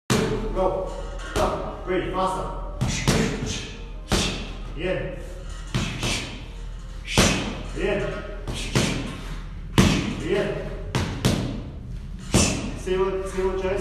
Pad work boxing cool kids training session
Some great pad work, these boys have excellent coordination and skill

We love to encourage our kids to be fit and healthy and get outdoors
Brothers Joseph and George have found a passion in boxing, boxing has many benefits for kids
It teaches them coordination, focus, strength, confidence, self defence and setting goals

There is much more to come!!
Thanks so much everyone we appreciate it and we hope we have helped encouraged your family to a healthy life style